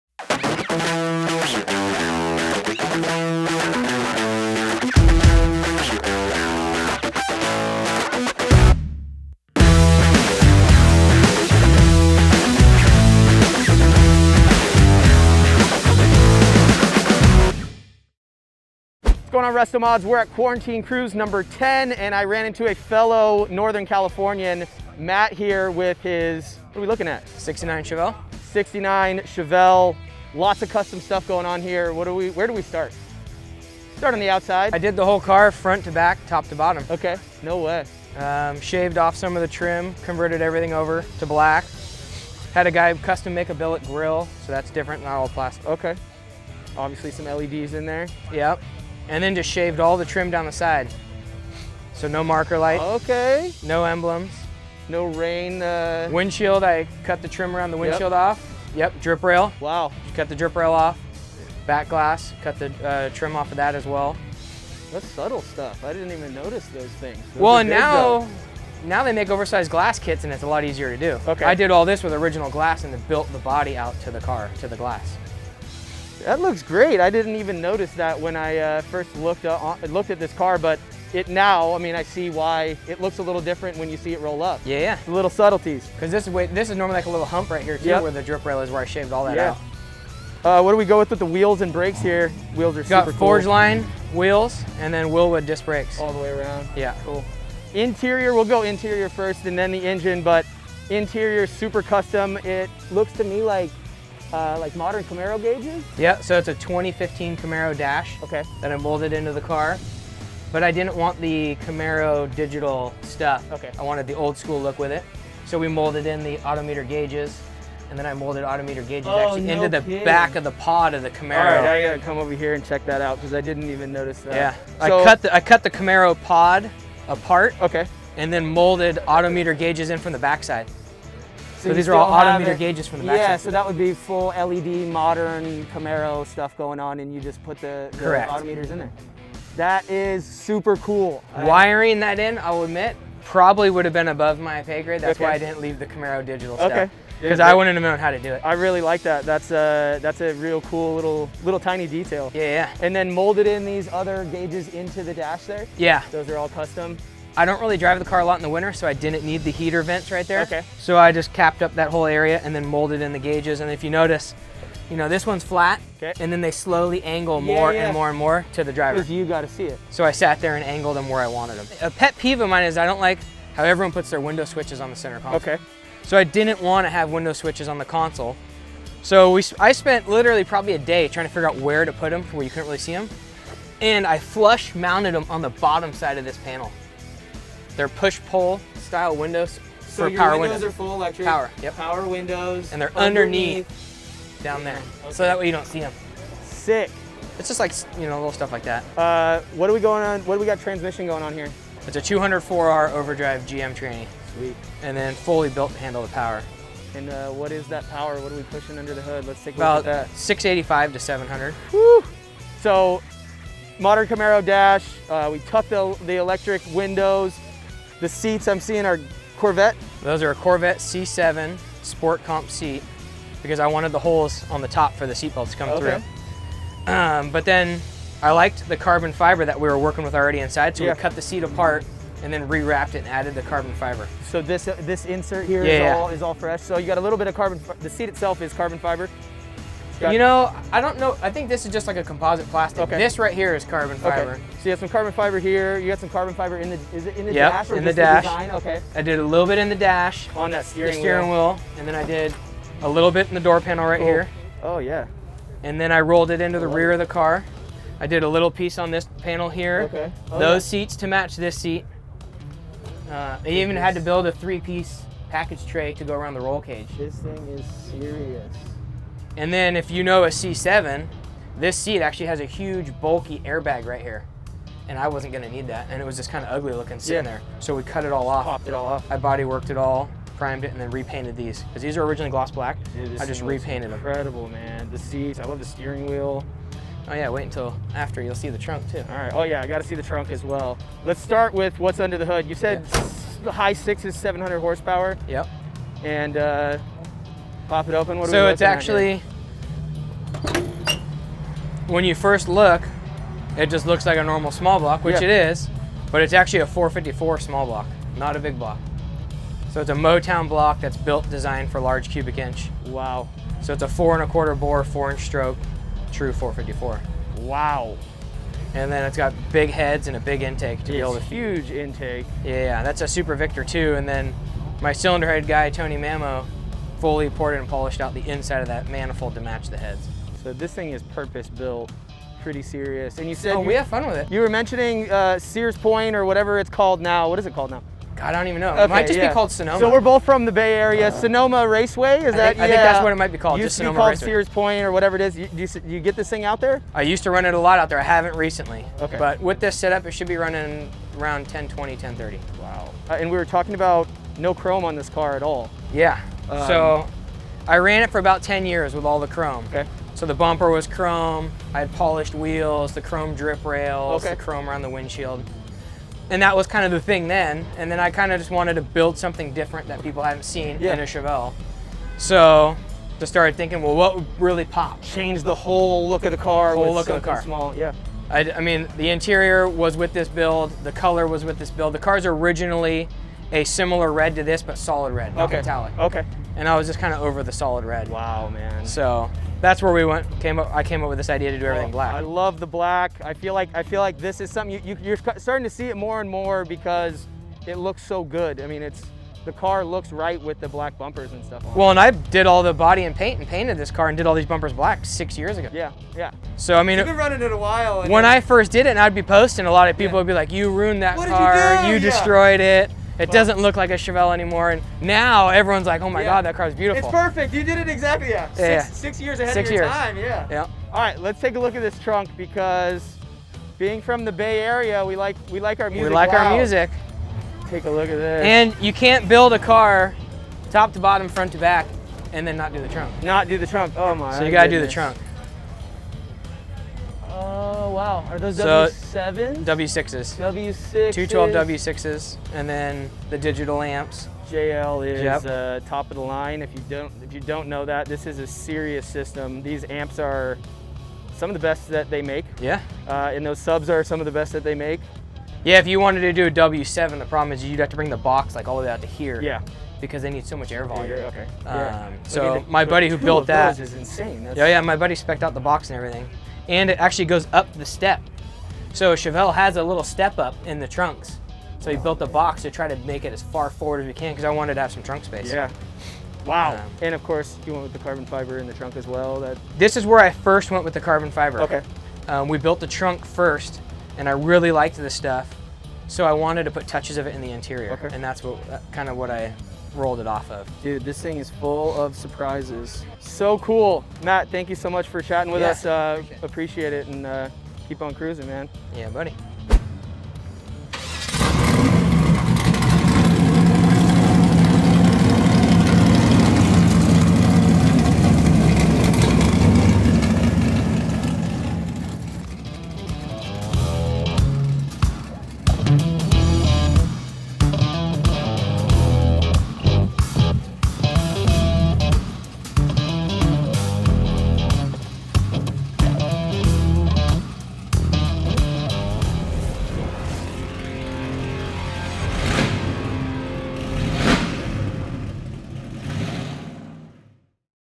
It Doesn't Get Much Better Than This '69 Chevelle Restomod | RestoMods Features
Matt brought his '69 Chevelle restomod to the Quarantine Cruise #10 and when we talked to him about it, the story on this build just kept getting more awesome. Lots of custom work has been done to this car, most of which was modified by Matt himself. Under the hood? a 700 horsepower small block bored and stroked to 454ci. With big heads and a big intake, plus plenty of other power upgrades, this Chevelle would've been a scary driving experience had it not been for the 4 wheel disc Wilwoods and coilover suspension all the way around. For a classic, this is probably about the best modern driving experience you can get thanks to Matt's vision of building a badass restomod Chevelle.

Get your chance to win a restomod of your own over